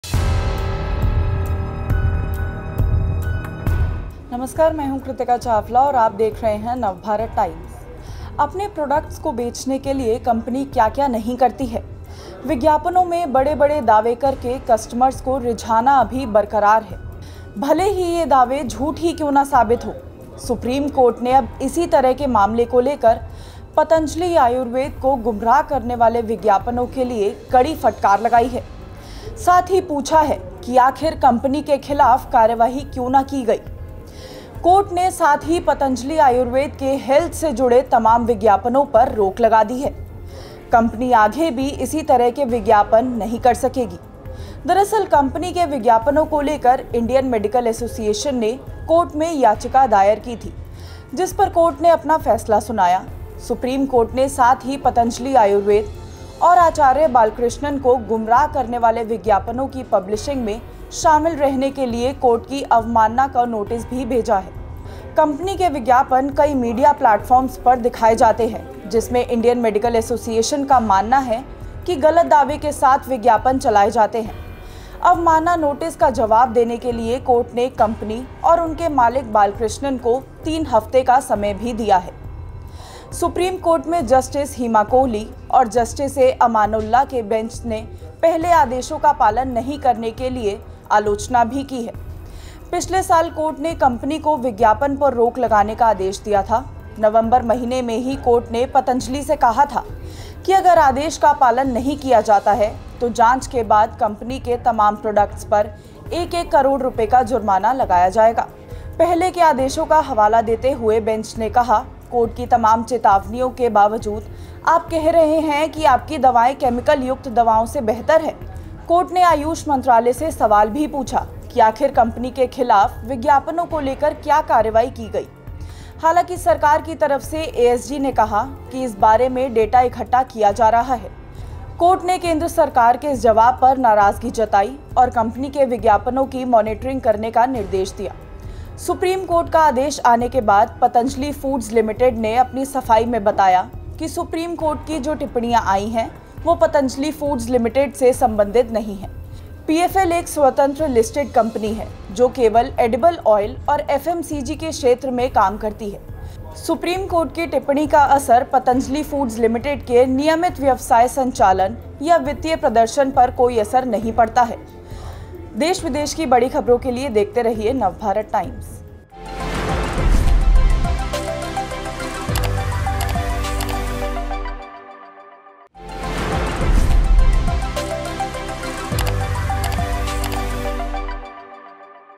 Supreme Court On Patanjali Misleading Ads Case: अब SC ने पतंजलि को क्यों लगाई फटकार? | NBT
अपने प्रोडक्ट को बेचने के लिए कंपनी क्या-क्या नहीं करती हैं. विज्ञापनों में बड़े-बड़े दावे करके कस्टमर्स को रिझाना अभी भी बरकरार है. भले ही ये दावे झूठे की क्यों न साबित हों.सुप्रीम कोर्ट ने अब इसी तरह के मामले को लेकर पतंजलि आयुर्वेद को "गुमराह करने वाले" विज्ञापनों के लिए कड़ी फटकार लगाई है. साथ ही पूछा कि आखिर कंपनी के खिलाफ कार्रवाई क्यों नहीं की गई. कोर्ट ने साथ ही पतंजलि आयुर्वेद के हेल्थ से जुड़े तमाम विज्ञापनों पर रोक लगा दी है.